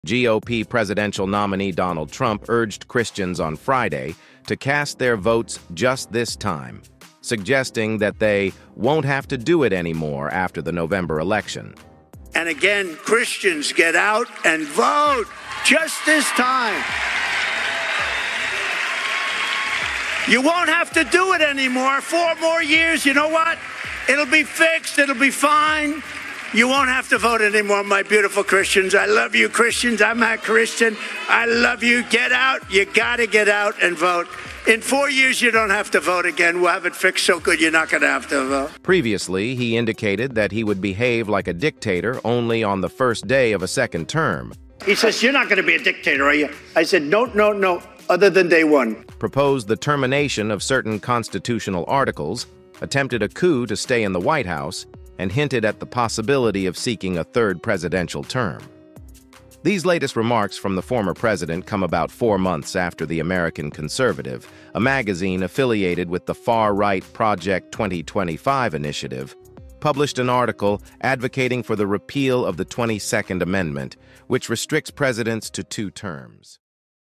Revealed: Trump's Final Promise to Christian Voters
Donald Trump urges Christians to cast their votes just this time, promising that they won't have to do it anymore after the November election. In this video, we discuss Trump's remarks, his plans for the future, and the implications for the Christian community.